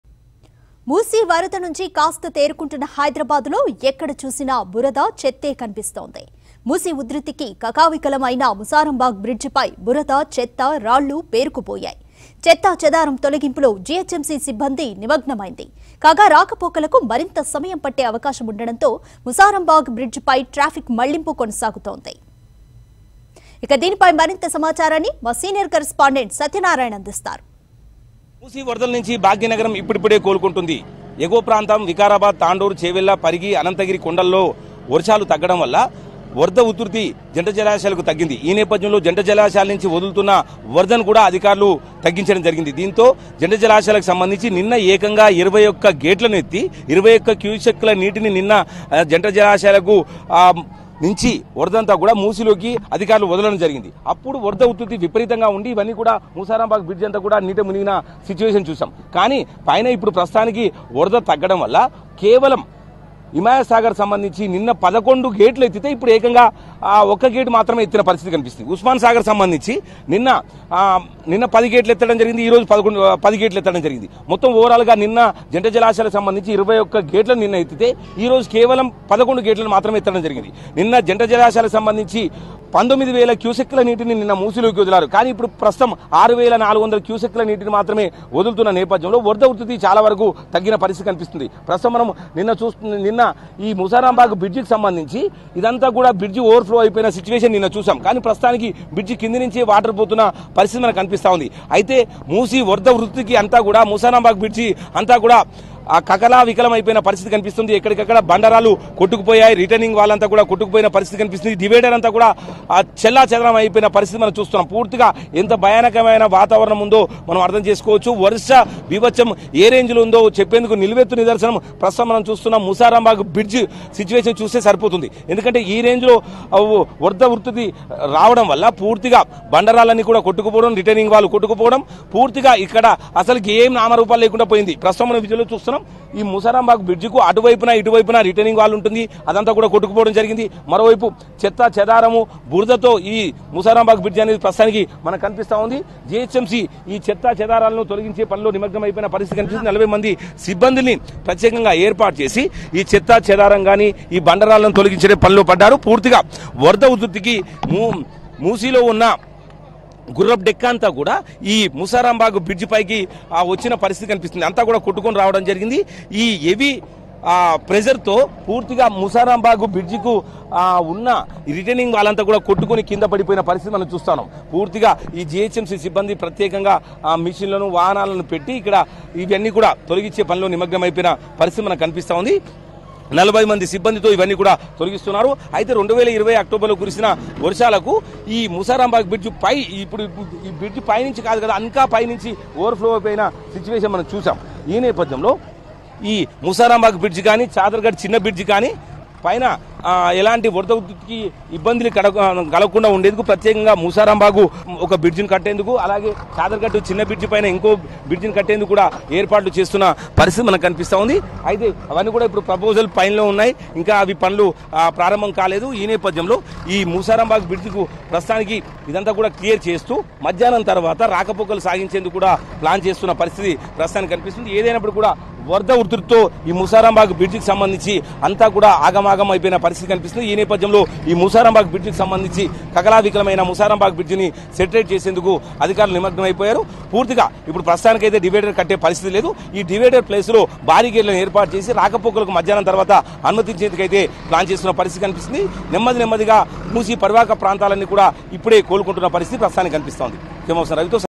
నామరూపాల్లేకుండా మూసారాంబాగ్ బ్రిడ్జి పరిస్థితి | Moosarambagh Bridge | Hyderabad Flood | TV5 News
మూసీ నది వరద ఉధృతికి నామరూపాల్లేకుండా
మూసారాంబాగ్ బ్రిడ్జి పరిస్థితి